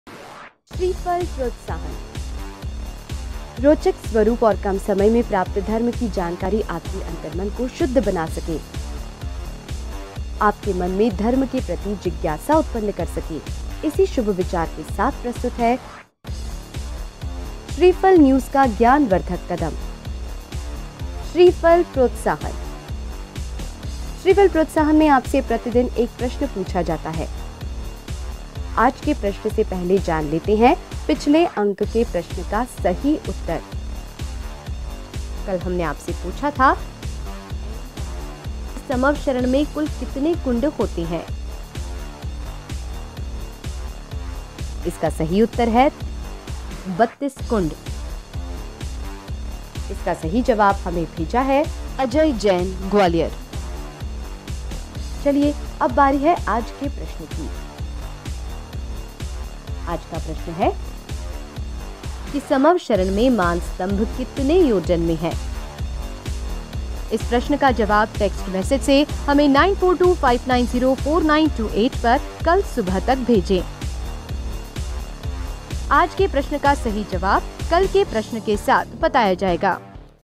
Shreephal protsahan# 15 -12 -2016# श्रीफल प्रोत्साहन # part 152
Shreephal protsahan# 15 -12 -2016# श्रीफल प्रोत्साहन # part 152 me deke aaj ka sawal aur part 151 ka sahi jawab aur vijeta ka naam